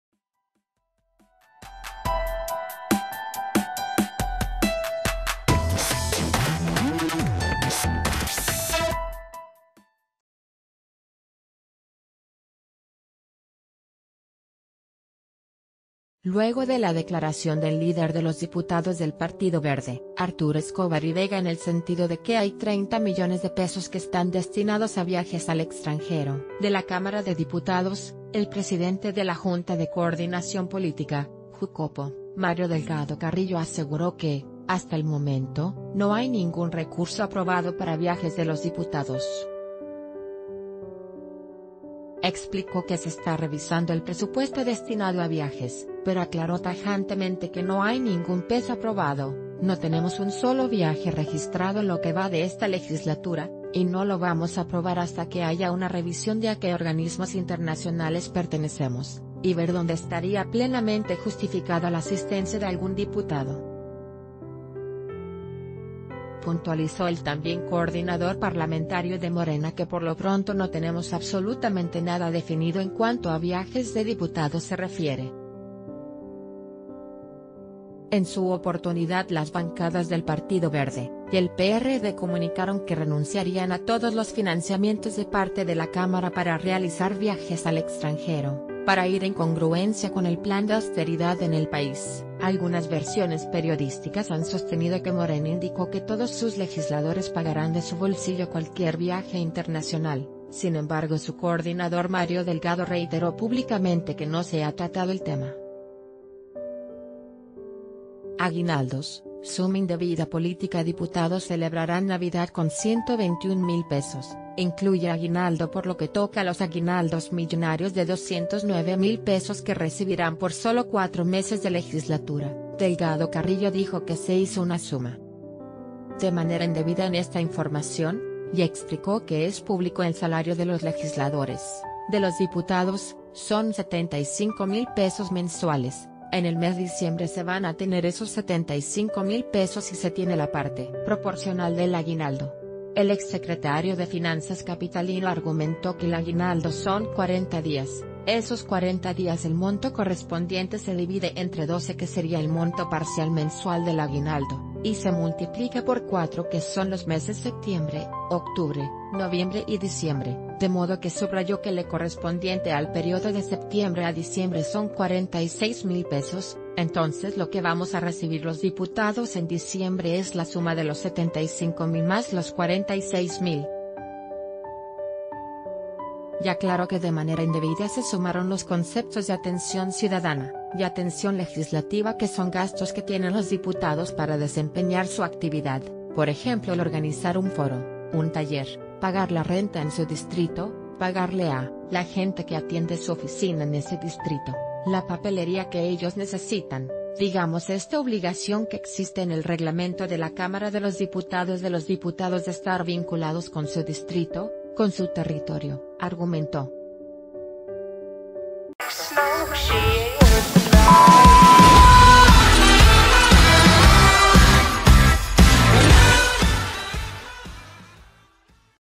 Diputados se contradicen por recursos para viajes internacionales
Luego de la declaración del líder de los diputados del Partido Verde, Arturo Escobar y Vega en el sentido de qué hay 30 millones de pesos que están destinados a viajes al extranjero de la Cámara de Diputados, el presidente de la Junta de Coordinación Política (Jucopo) Mario Delgado Carrillo aseguró ...